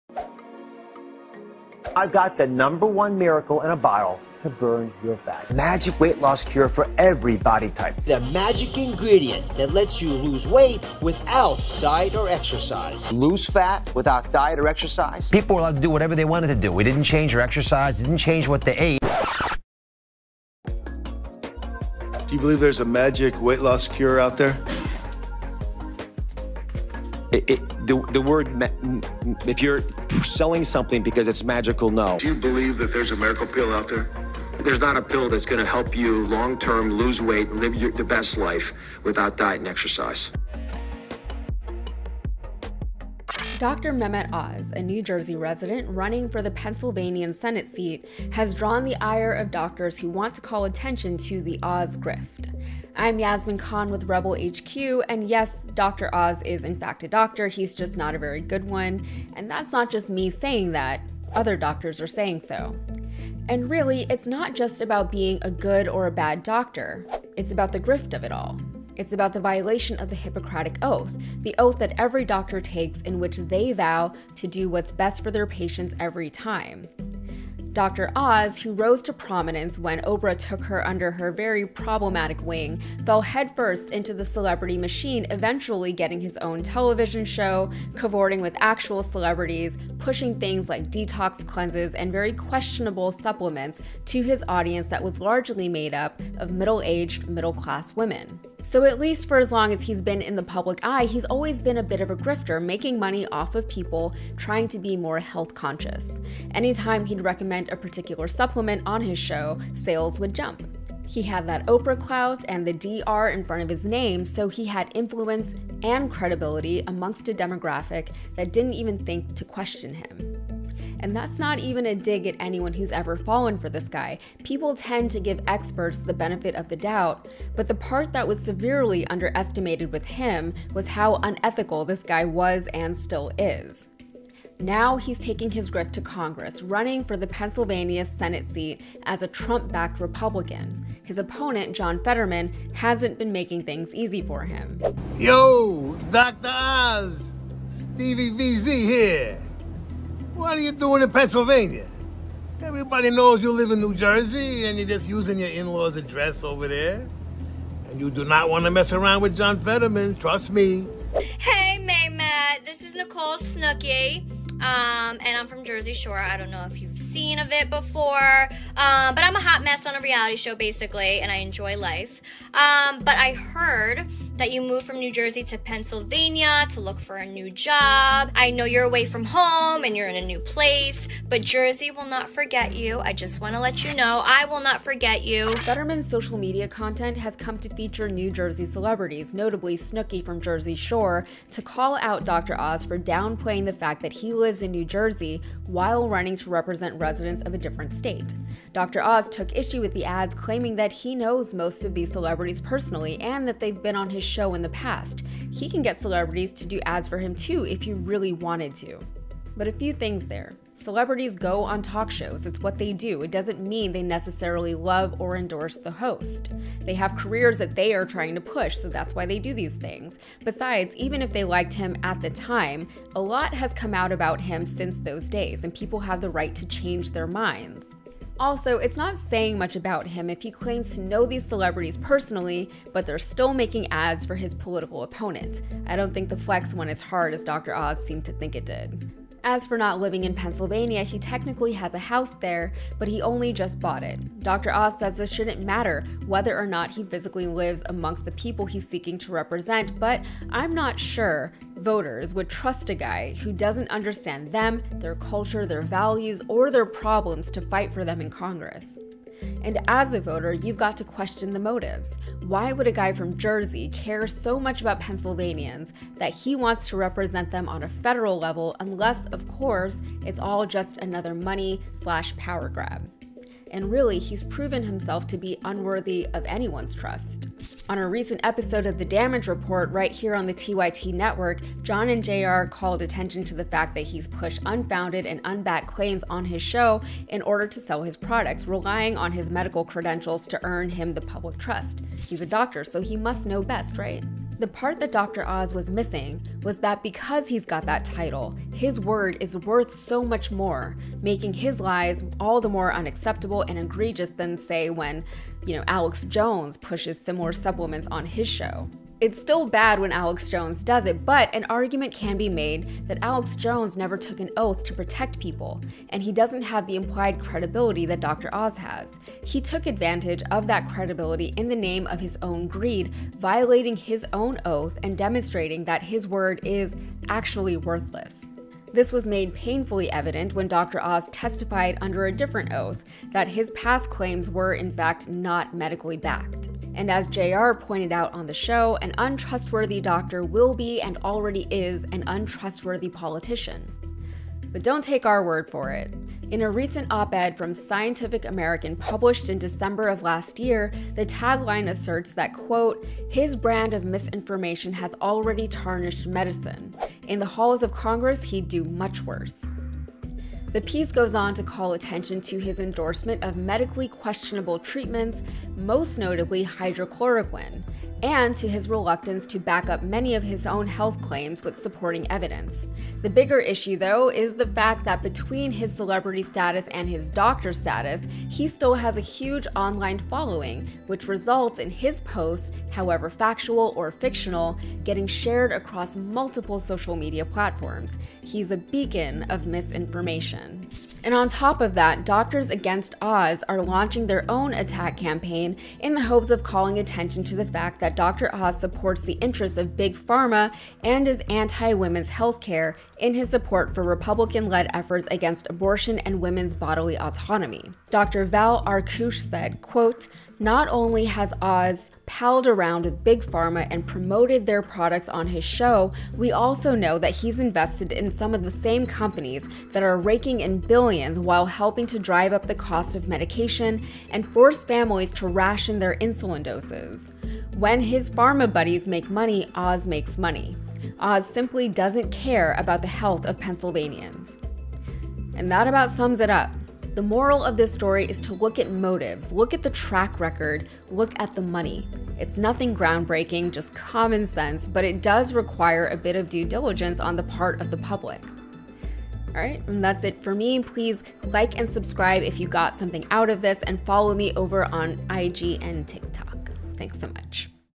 Dr. Oz Gets Exposed As A 'Quack' By REAL Doctors
Republican Senate Candidate and C-list daytime TV celebrity Dr. Mehmet Oz gets called out by a coalition of actual doctors. Doctors Against Oz have spent the summer denouncing the GOP candidate as a 'Quack'. Yasmin Khan breaks it down on Rebel HQ.